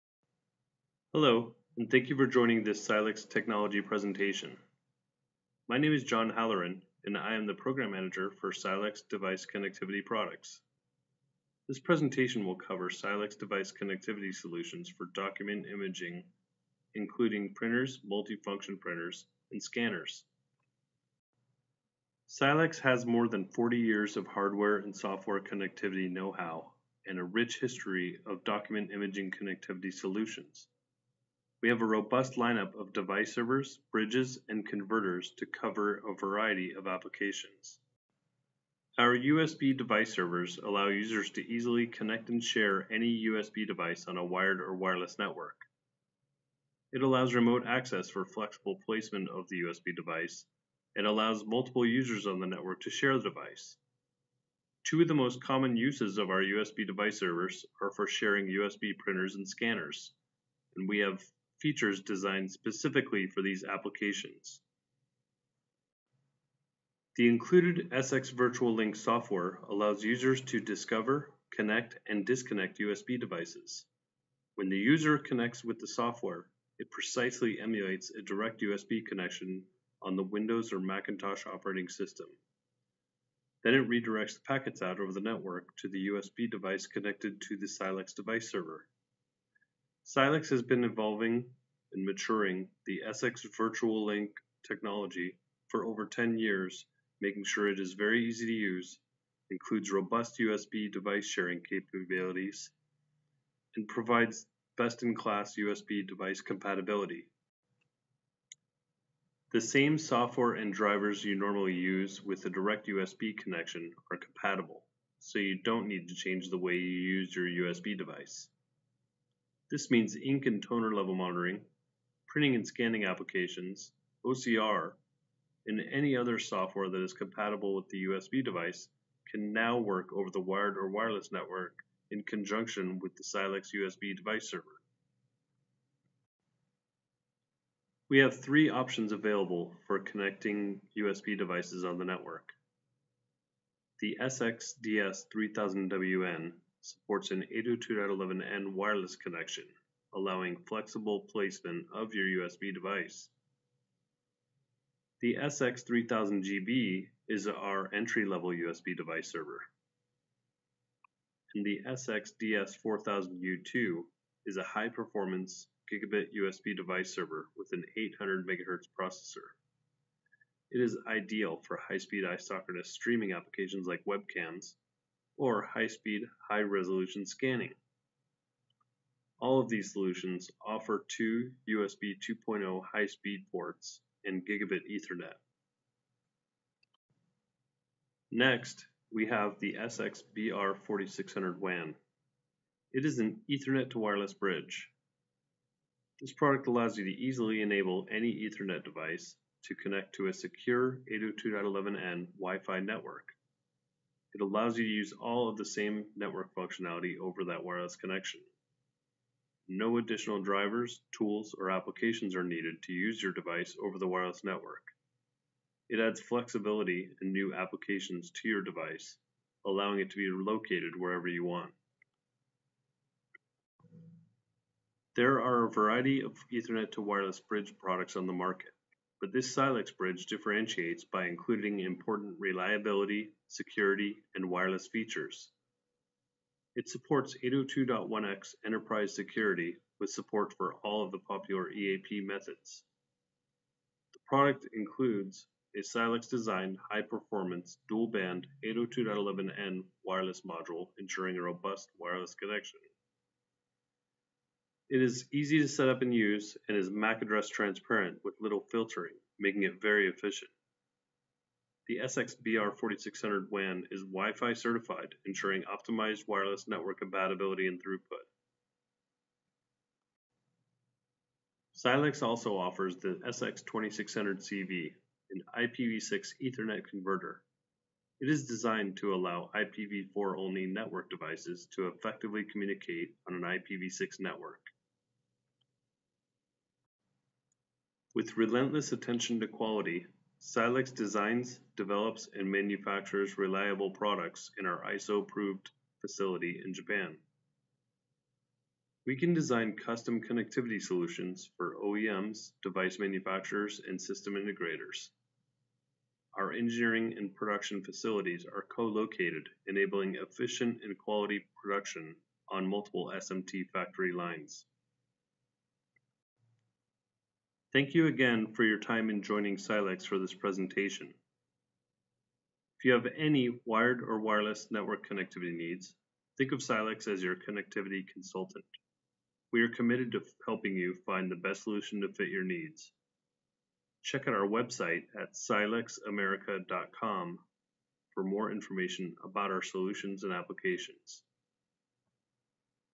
Silex Device Connectivity Solutions for Document Imaging
This slide-cast goes over all the device connectivity solutions (USB to wireless, USB device server, Ethernet to wireless and serial to Wi-Fi) for document imaging. It includes printers, multifunction printers and scanners.